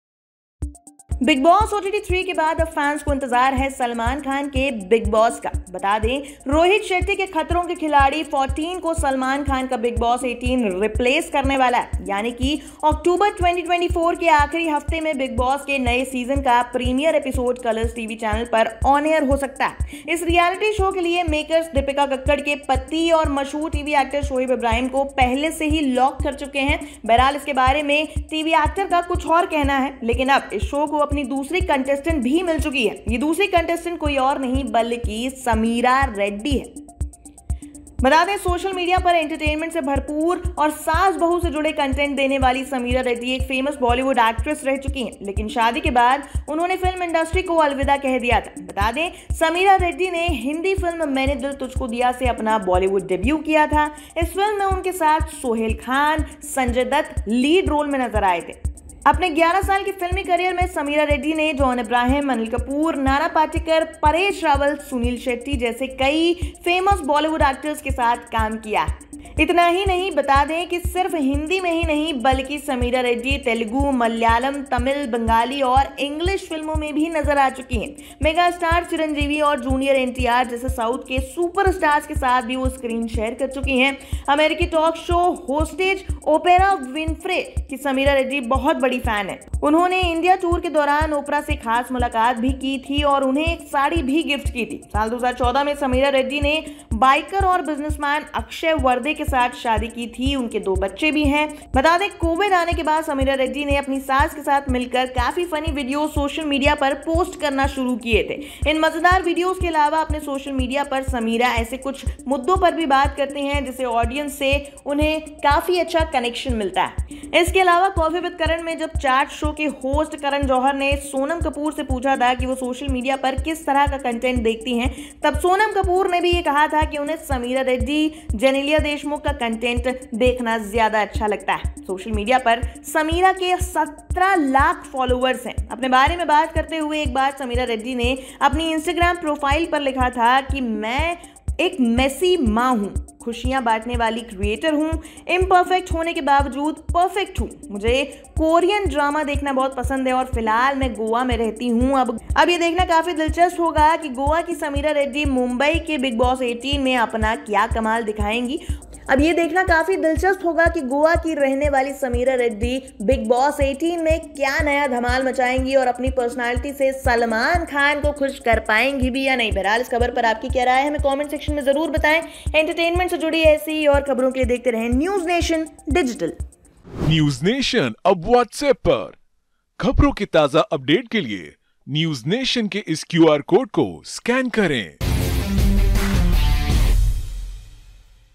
Bigg Boss 18 में जलवा बिखेरेगी ये हॉट हसीना Salman Khan की है फेवरेट |Entertainment
Bigg Boss 18 को लेकर नया अपडेट सामने आया है. अब सलमान खान के शो की नई कंटेस्टेंट का नाम लीक हो चुका है. वो कोई और नहीं हैं बल्कि बॉलीवुड एक्ट्रेस Sameera Reddy है.

A new update has come out regarding Bigg Boss 18. Now the name of the new contestant of Salman Khan's show has been leaked. She is none other than Bollywood actress Sameera Reddy.